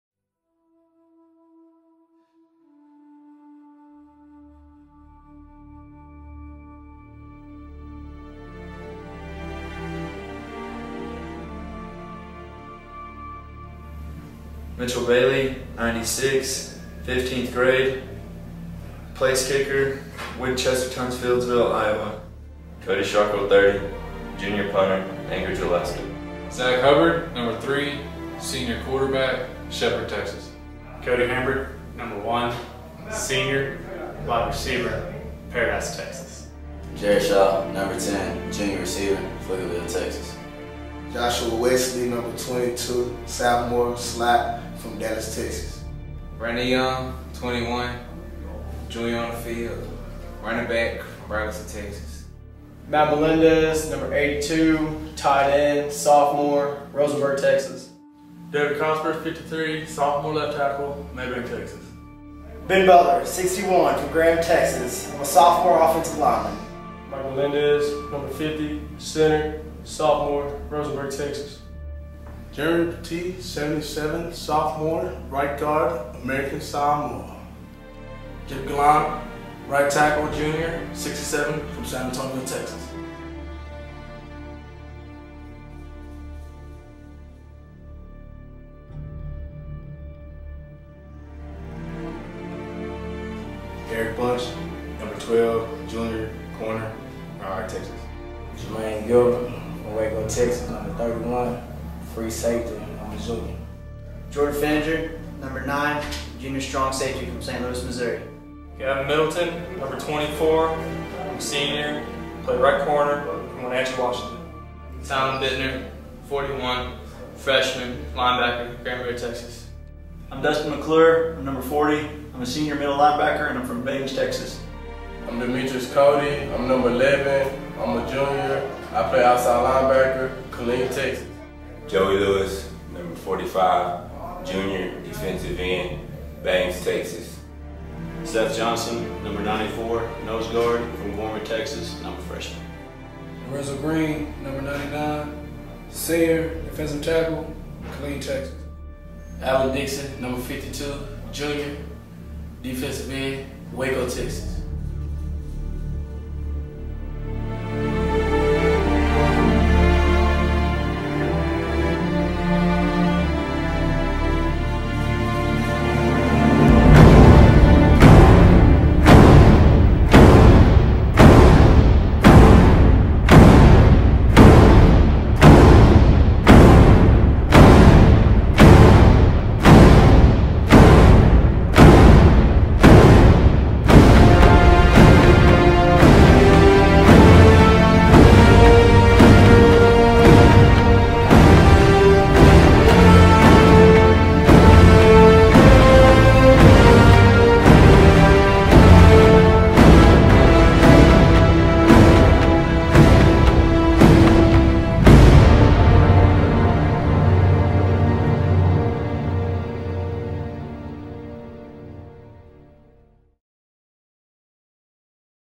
Carlton Entertainment presents: 2010 Starting Lineups
Starting lineup video to play on the jumbotron before every home HPU football game